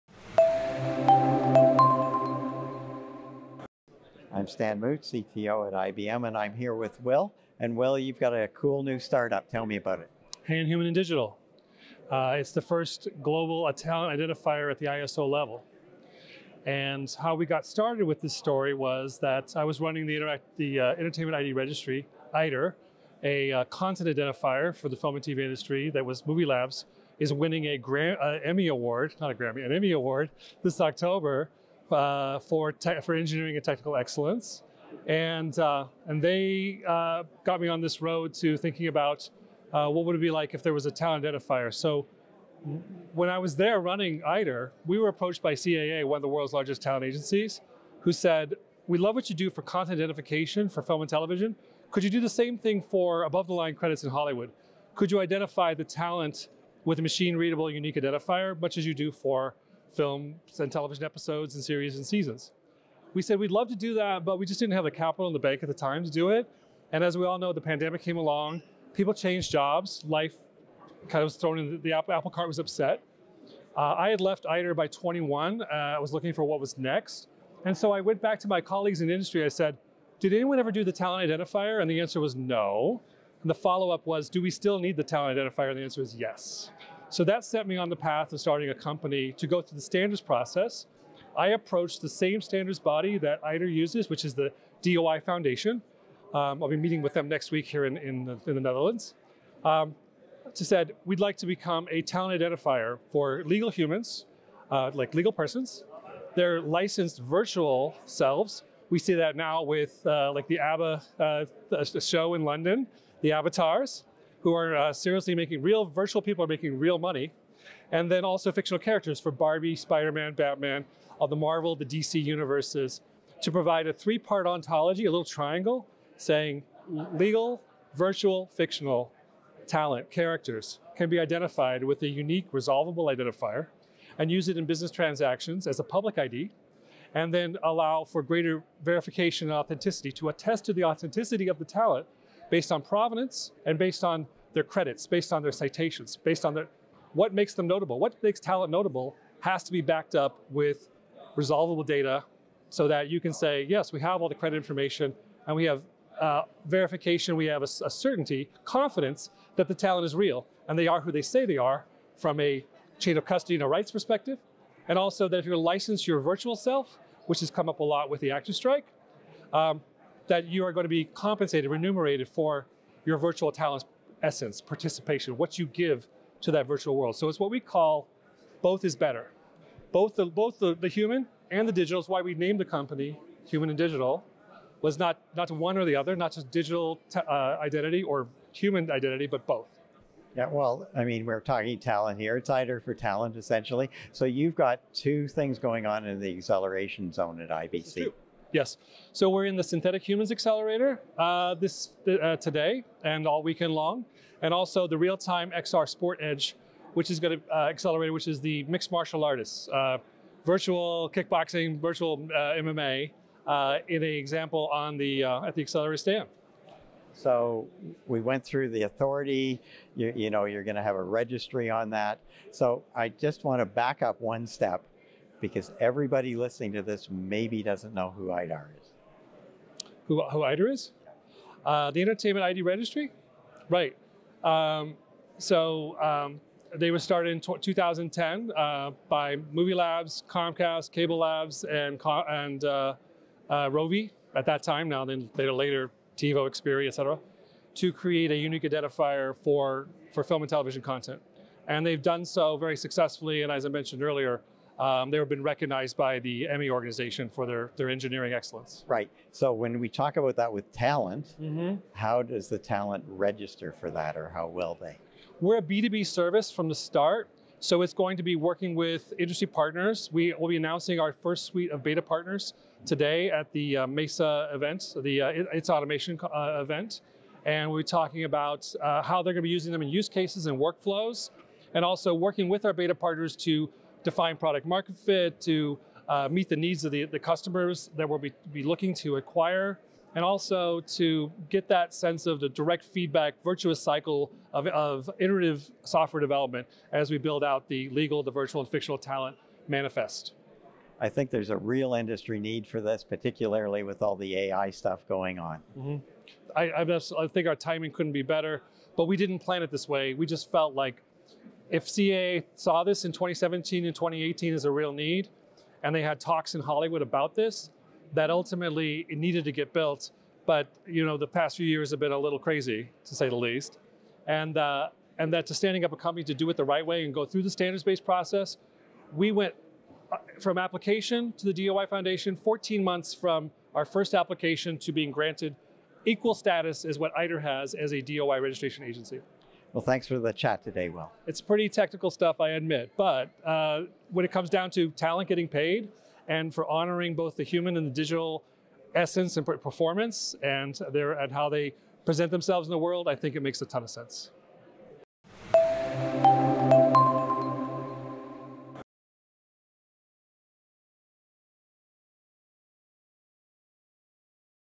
In Conversation with HAND at IBC 23
Talent is going Virtual – and Virtual Talent needs provenance automation for ID security, measurement, and compensation. In this IABM TV interview, Will Kreth, CEO of HAND (Human & Digital), discusses HAND’s interoperable Talent ID as DOI.org ID registry – and the launch their Beta Partner Program.